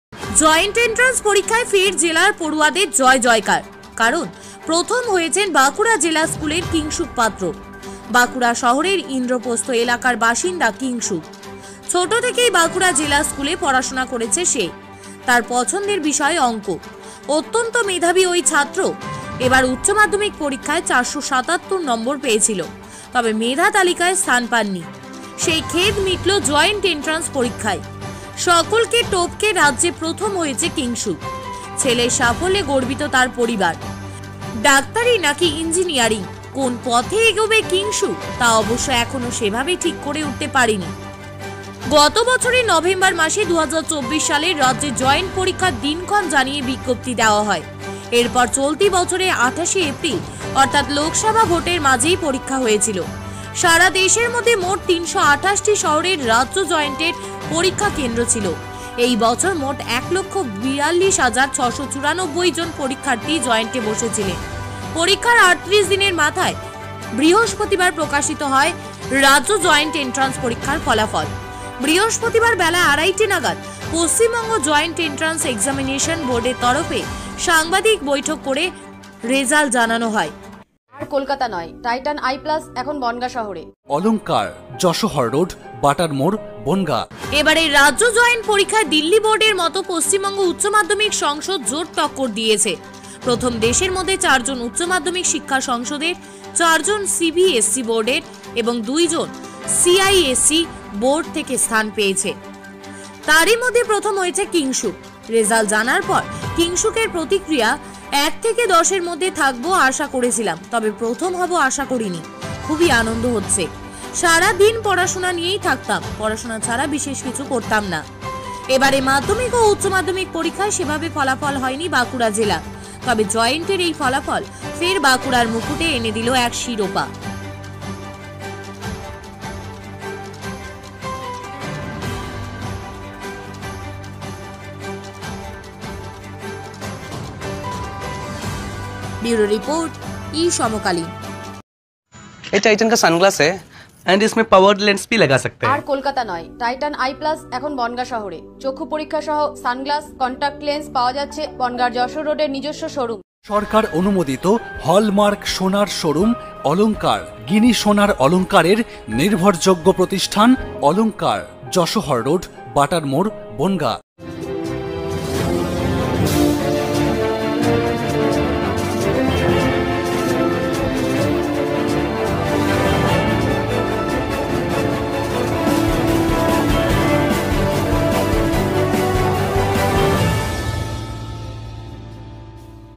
জয়েন্ট এন্ট্রান্স-এ প্রথম বাঁকুড়ার কিংশুক - Kingshuk of First Bankura at Joint Entrance
জয়েন্ট এন্ট্রান্স-এ প্রথম বাঁকুড়ার কিংশুক
Kingshuk of First Bankura at Joint Entrance

জয়েন্ট এন্ট্রান্স পরীক্ষায় ফের জেলার পড়ুয়াদের জয়জয়কার। কারণ, প্রথম হয়েছেন বাঁকুড়া জেলা স্কুলের কিংশুক পাত্র। বাঁকুড়া শহরের ইন্দ্রপ্রস্থ এলাকার বাসিন্দা কিংশুক। ছোট থেকেই বাঁকুড়া জেলা স্কুলে পড়াশোনা করেছে সে। তাঁর পছন্দের বিষয় অঙ্ক। অত্যন্ত মেধাবী ওই ছাত্র এবার উচ্চ মাধ্যমিক পরীক্ষায় ৪৭৭ নম্বর পেয়েছিল। তবে মেধা তালিকায় স্থান পাননি। সেই খেদ মিটল জয়েন্ট এন্ট্রান্স পরীক্ষায়। সকলকে টপকে রাজ্যে প্রথম হয়েছে কিংশুক। ছেলের সাফল্যে গর্বিত তাঁর পরিবার। ডাক্তারি নাকি ইঞ্জিনিয়ারিং- কোন পথে এগোবে কিংশুক, তা অবশ্য এখনও সেভাবে ঠিক করে উঠতে পারেনি।